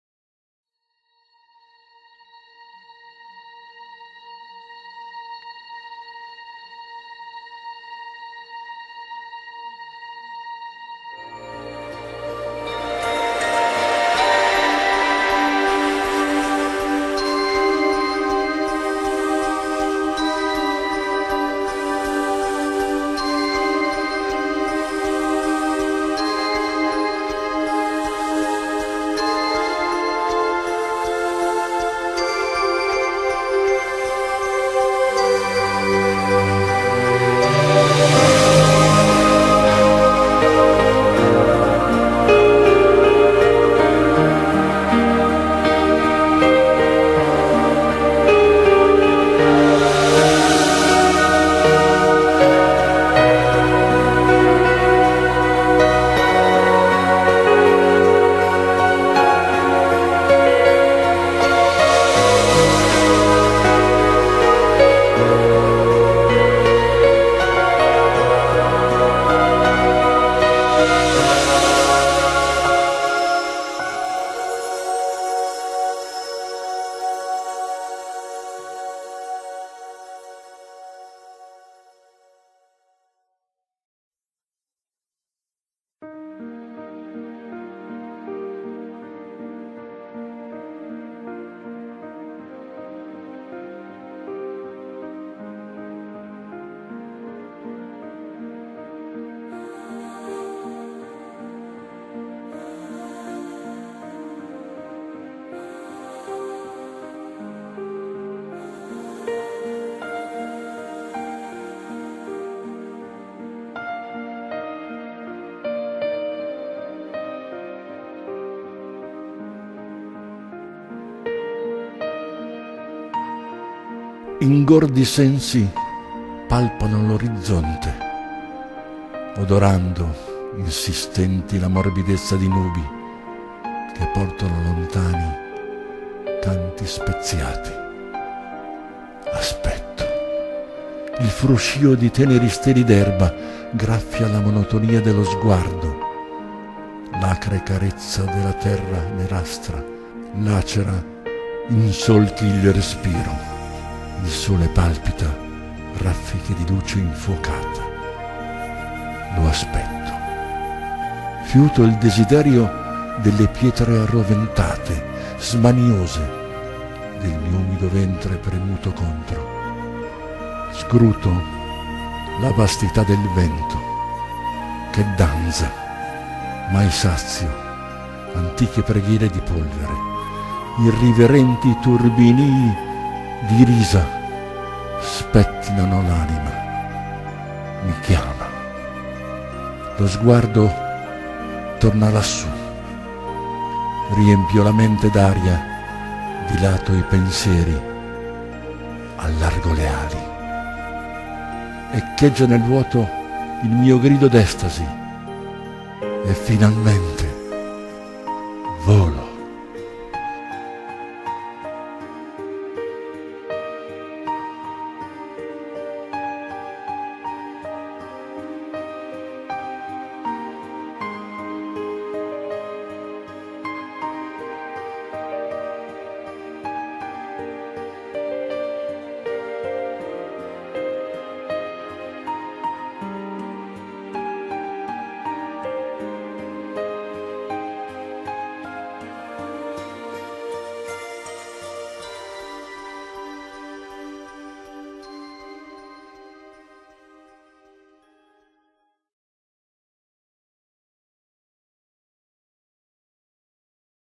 Il mio grido Manuela Canicattì - Video poesia Massimo Rotundo
8 GIUGNO 2014
V Edizione del Premio Nazionale di Poesia "IO ESISTO"
PALAZZO MEDICEO OTTAVIANO (NA)

Organizzato dalla U.I.L.D.M.
Unione Italiana Lotta alla Distrofia Muscolare
Sezione di Ottaviano (Na)

3° CLASSIFICATO
MANUELA CANICATTI'
   "IL MIO GRIDO"

Il mio grido Manuela Canicattì - Voce Nino Castorina - Musica Marc Enfroy - Video poesia Massimo Rotundo.

IL MIO GRIDO

Ingordi sensi palpano l'orizzonte
odorando insistenti la morbidezza di nubi
Che portano lontani canti speziati.

Aspetto .

Il fruscio di teneri steli d'erba
graffia la monotonia dello sguardo;
l'acre carezza della terra nerastra
lacera in solchi il respiro .
Il sole palpita raffiche di luce infuocata.

Lo aspetto.

Fiuto il desiderio delle pietre arroventate
smaniose del mio umido ventre premuto contro
Scruto la vastità del vento
che  danza mai sazio antiche preghiere di polvere.
Irriverenti turbinii di risa
spettinano l'anima.

Mi chiama!

Lo sguardo torna lassù...
Riempio la mente d'aria
Dilato i pensieri
Allargo le ali.
Echeggia nel vuoto il mio grido d'estasi
e finalmente volo.